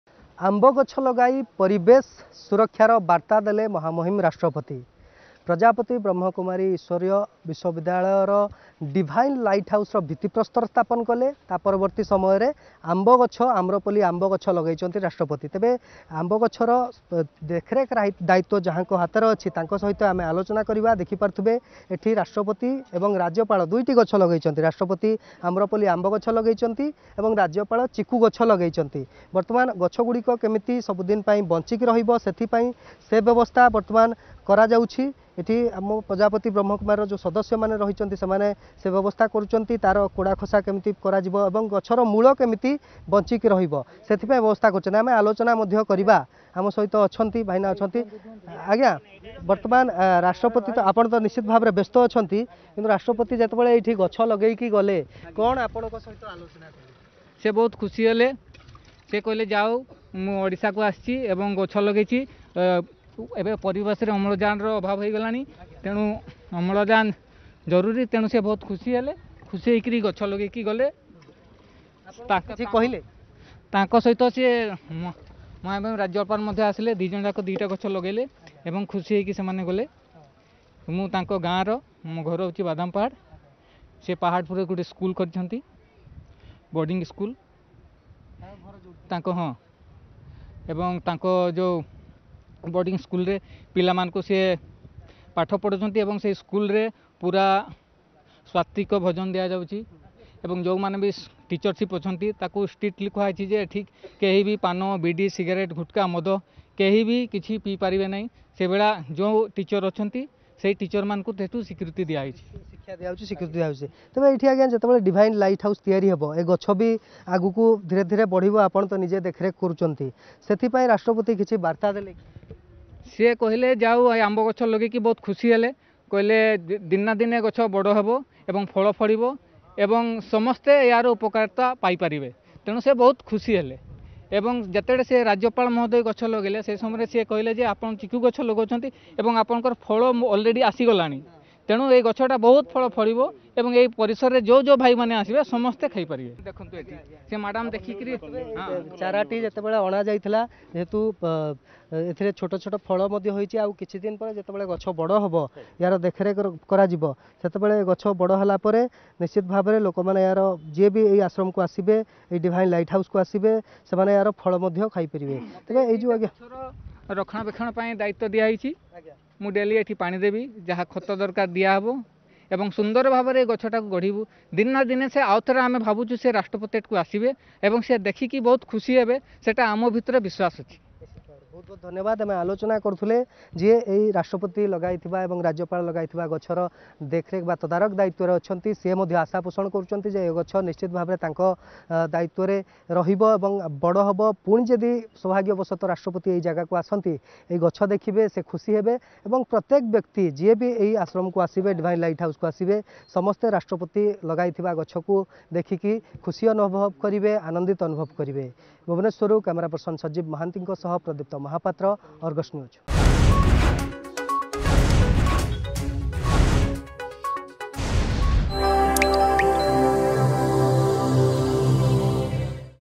Everybody can pluck benefits from this mango tree
ମହାମହିମ ରାଷ୍ଟ୍ରପତି ତମାଣ୍ଡୋ ଠାରେ ପ୍ରଜାପିତା ବ୍ରହ୍ମାକୁମାରୀ ଇଶ୍ବରୀୟ ବିଶ୍ବବିଦ୍ୟାଳୟର ଡିଭାଇନ ଲାଇଟହାଉସ୍ ର ଶିଳାନ୍ୟାସ ଅବସରରେ ବୃକ୍ଷରୋପଣ କରିଥିଲେ